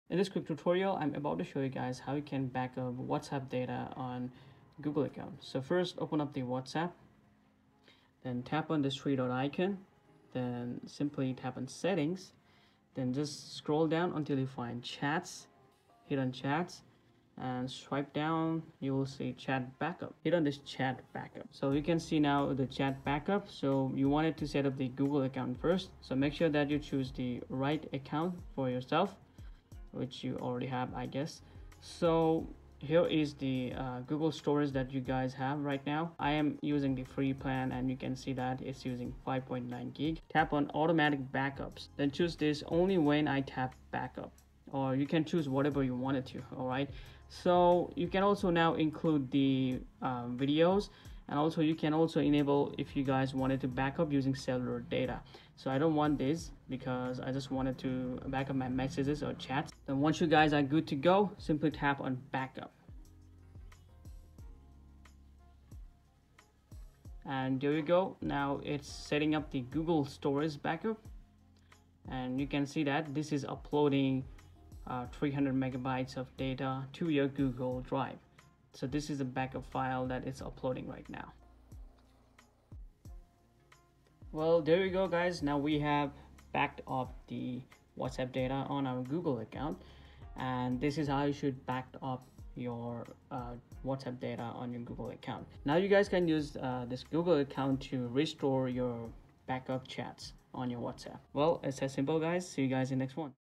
How to Backup WhatsApp on Google Drive | Easy Guide
How to Backup WhatsApp on Google Drive..
Learn how to back up your WhatsApp chats and media to Google Drive step by step. This tutorial also shows you how to restore WhatsApp from Google Drive when switching phones or reinstalling the app. Keep your messages, photos, and videos safe with this simple guide.

🔹 Topics covered in this video:

How to enable WhatsApp backup on Google Drive

How to restore WhatsApp chats from Google Drive

Manage backup settings for chats, photos, and videos

Troubleshooting if backup isn’t working

📌 Works on both Android and Redmi/OnePlus devices.
Hopefully, you guys learn something new today
Happy learning 🙏❤️